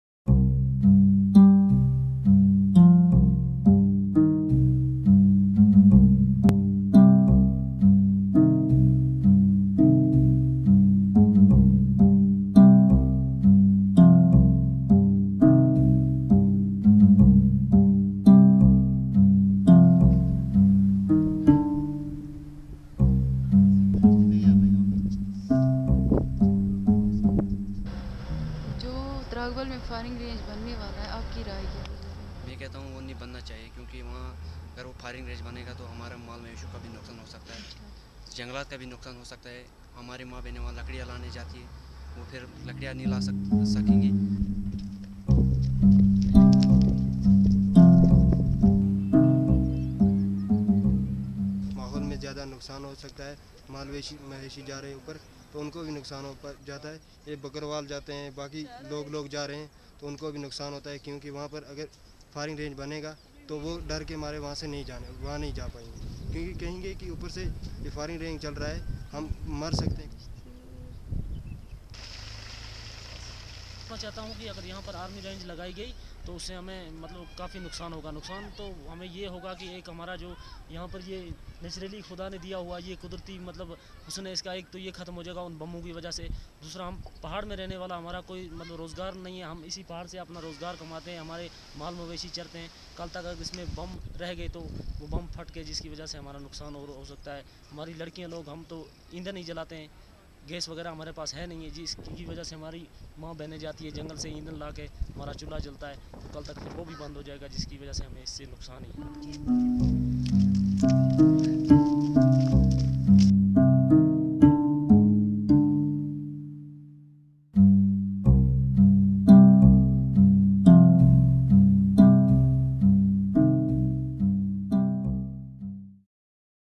Proposed Tragbal Firing range Threatens livelihood of Hundreds of Shepherds in Bandipora
The Government of Jammu and Kashmir has been planning to provide Tragbal meadows in north Kashmir’s Bandipora district to the Army for setting up an artillery firing range, a replacement of the Tosa Maidan field firing range which was vacated by the army after a collective movement by several rights groups and locals.
Earlier, the authorities had decided to hand over land at Tulail in the Gurez area of the district Bandipora which was dropped after taking into consideration the severe damage it could lead to the environment of that area. The Tragbal firing range lies adjacent to the Razdan pass which connects the Gurez with rest of the Kashmir valley. Locals fear that the ammunition used for practice on the firing range will not only affect the environment but will take a direct hit on their livelihood. Aneesa Hamid reports from Bandipora